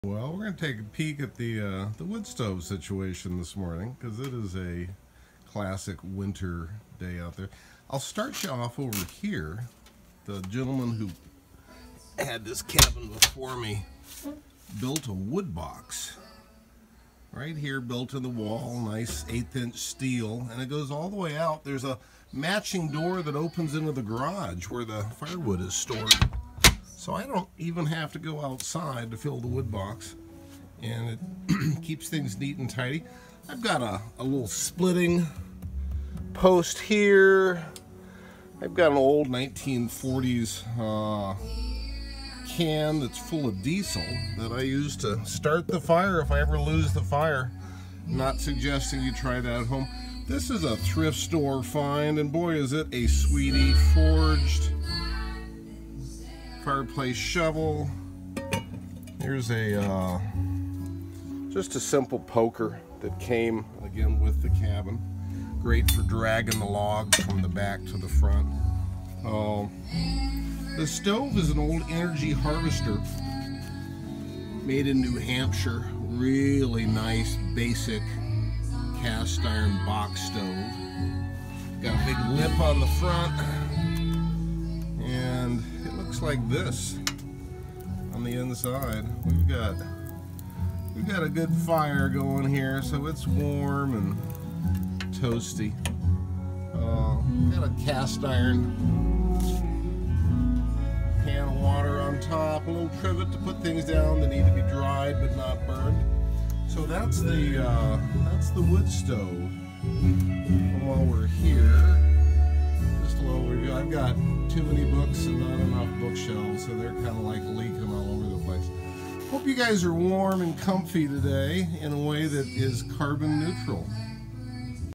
Running The Wood Stove 1.10.2023
My cabin heats with wood.  Carbon neutral, and emotionally comforting at a very primal level.  Heating with wood is a lot of work, even if you buy your firewood already split, which I have done for the past several years.  But that work is as satisfying as sitting by the stove on a cold winter morning.

"Wood heats you twice" is the quote, referring to the sweat you get from the cutting and stacking in the summer.  I like to have my wood put up in April, if possible, when it's still pretty cool outside, and when the wood has all summer to dry out a bit and develop some of the checks that show it is ready to burn.  I'm lucky here in the cabin, with room in the garage to store the three cords of wood needed for a winter, and a clever iron wood box that joins the garage to the inside of the cabin, right next to the wood stove.  I load up the box with about thirty splits, maybe once a week when it's not too cold, and maybe every three days when the real chill is here.

The wood stove is a 1970s Energy Harvester, made in New Hampshire/Massachusetts, by the same kind of pot smoking hippies that started Vermont Castings around the same time.  Vermont Castings had better marketing, maybe, but the Energy Harvester is a better stove, in my view.  Made of thick C-60 cast iron, and joined well enough to never need rebuilding.  It's a simple box stove, like the early Jotuls and Morsos, on which it is based.  The logs burn cigar style, from one end to the other, leaving you with a nice pile of coals tucked in the very back of the stove, ready to start the fire the next morning.  It's not super efficient, but, boy! it is super easy to run and maintain.  This makes for a nice clean chimney, and no struggles in getting the wood to burn.

The wood stove is the heart of the cabin, and where I like to spend my time.

You might hear Nora Brown, my favorite young banjo magician, in the back ground.,  She is a favorite of mine, and well worth seeking out and listening to.  Such a channel to the music of a hundred years ago.  Nora Brown goes well with wood heat.